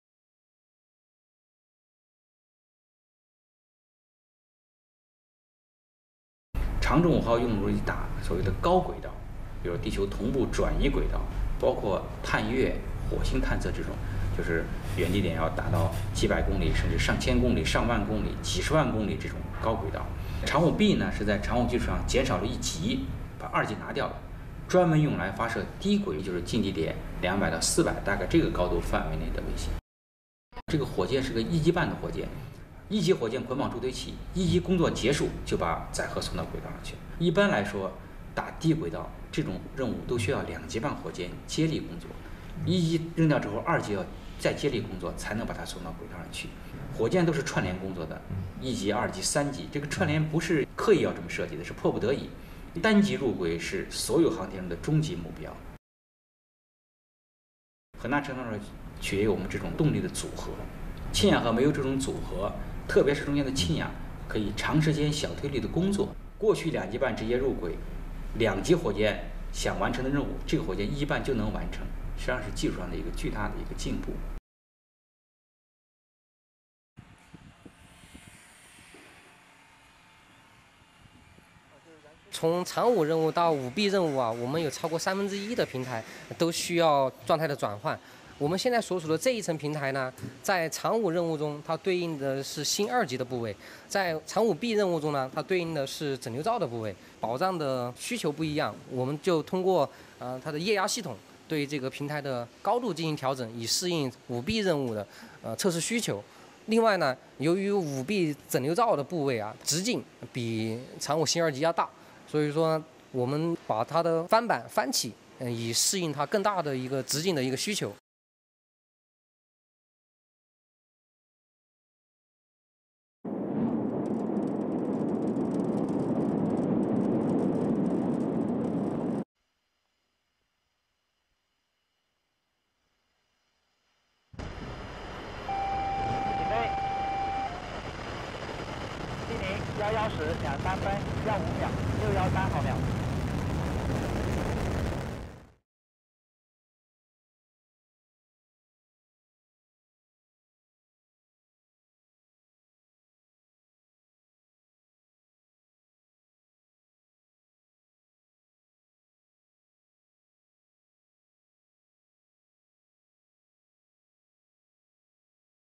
Long March-5B - the “one-and-half stage” rocket
The Long March-5B launch vehicle is a “one-and-half stage” rocket. Li Dong (chief designer of Long March-5, China Academy of Launch Vehicle Technology - CALT) explains the differences between Long March-5 and Long March-5B. The first Long March-5B rocket launched China’s new-generation crewed spacecraft on 5 May 2020.The second Long March-5B rocket launched the Tianhe core module of the China Space Station on 29 April 2021.
Credit: China Central Television (CCTV)